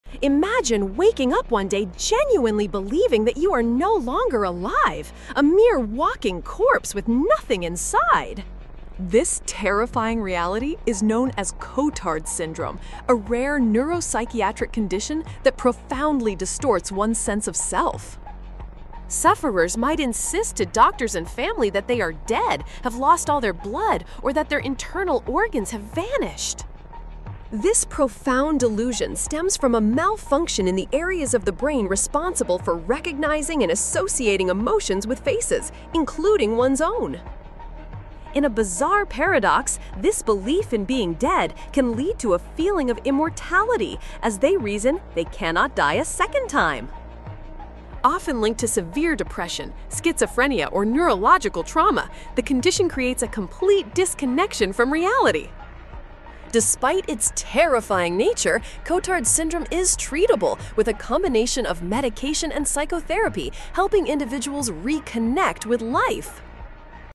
The Disease That Makes You Think You're Dead
Uncover the mysteries of Cotard's Syndrome, a rare neurological condition where individuals believe they are deceased, missing organs, or don't exist at all. This video explores the symptoms, causes, and treatment for this bizarre delusion.